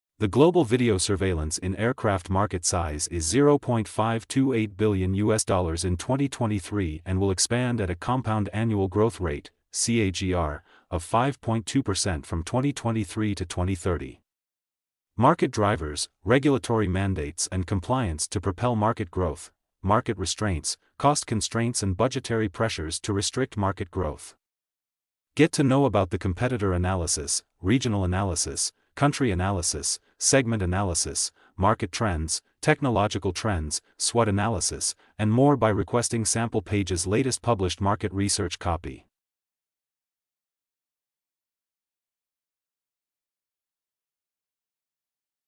Video Surveillance In Aircrafts Market Report | Forecast, Market Size & Growth
Report Insights: According to Cognitive Market Research, The Global Video Surveillance In aircraft market size is USD 0.528 billion in 2023 and will expand at a compound annual growth rate (CAGR) of 5.20% from 2023 to 2030.

The demand for Video Surveillance in aircraft is rising due to enhanced security measures and regulatory compliance.
Demand for Line Fit Surveillance Systems remains higher in the Video Surveillance in the aircraft market.
The Narrow Body Aircraft category held the highest Video Surveillance In aircraft market revenue share in 2023.
North American Video Surveillance in aircraft will continue to lead, whereas the Asia-Pacific Video Surveillance In aircraft market will experience the most substantial growth until 2030.